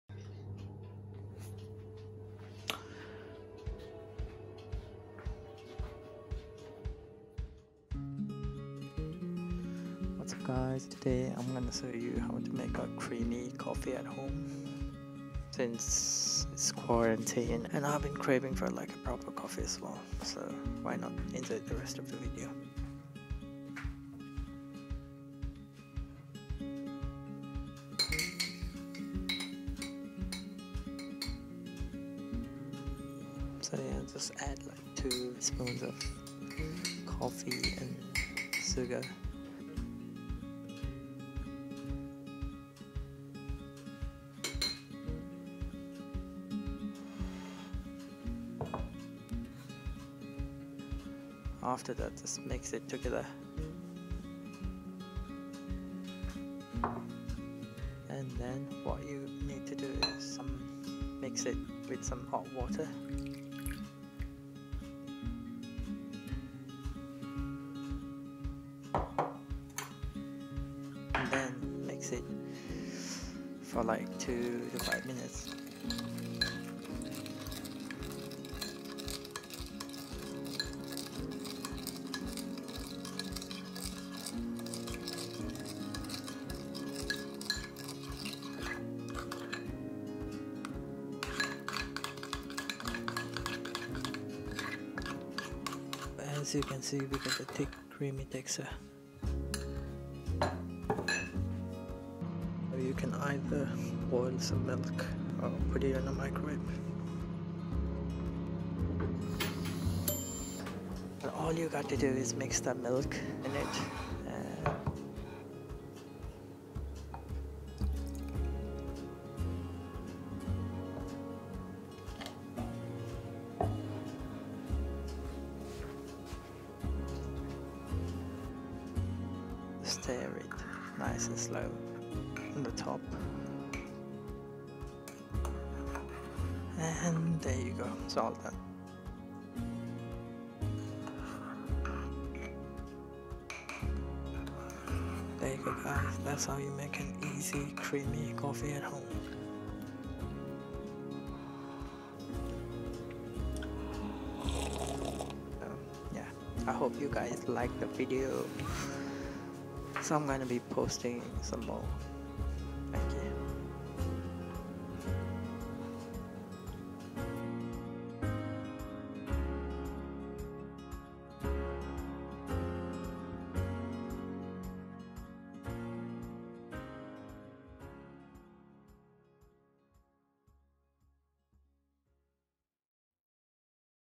HOMEMADE CREAMY COFFEE
Hi everyone since it's quarantine and everyone are staying at home. Here  is a short nice and easy video about homemade creamy coffee. you can follow these easy steps.